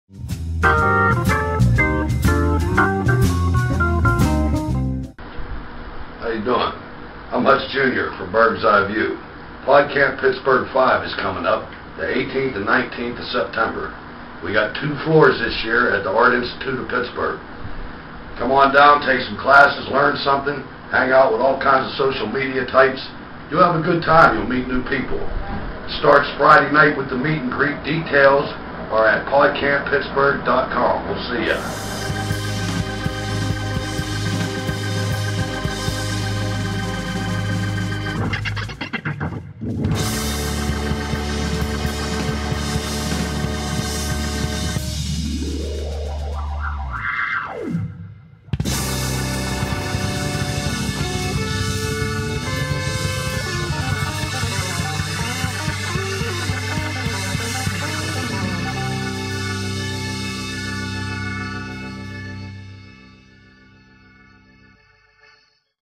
BurghseyeView: Go to Podcamp!
Hutch from the long running BurghCast, Burghseyeview, has some things to say about Podcamp Pittsburgh 5!